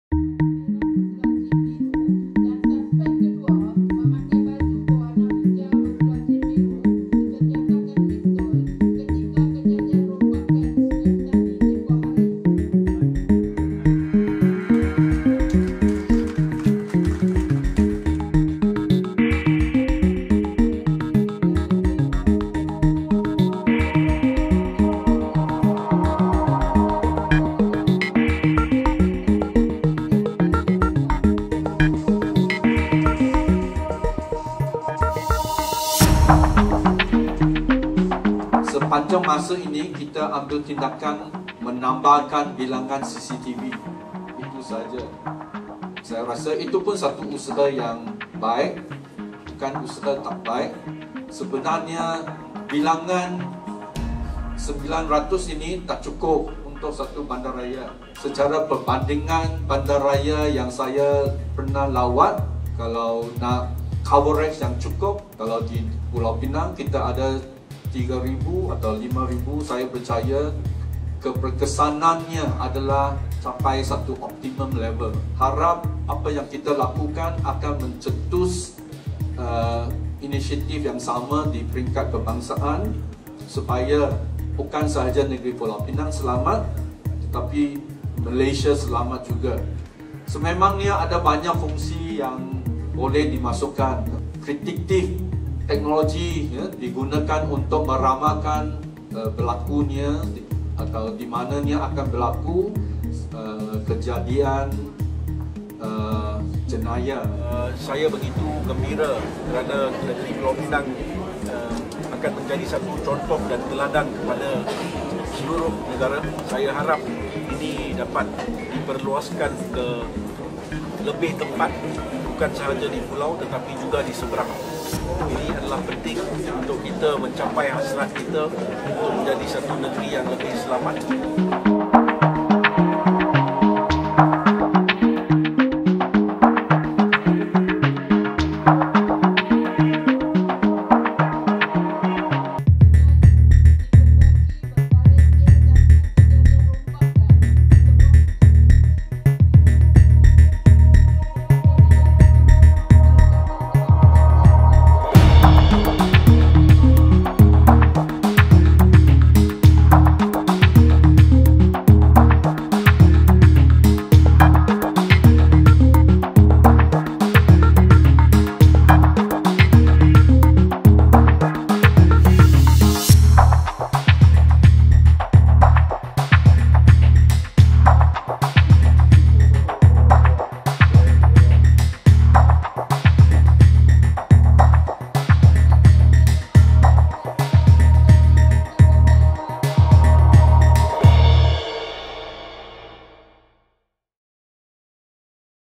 Facial recognition used on wanted individuals on Penang island
IndigoVision high resolution cameras with in built CyberVigilant technology, Distributed Network Architecture with no-single-point-of-failure, were able to integrate with 900 IndigoVision cameras on Penang Island to create a state-of-the-art Facial Recognition System that is used on wanted individuals. So criminals, you can run but you can't hide..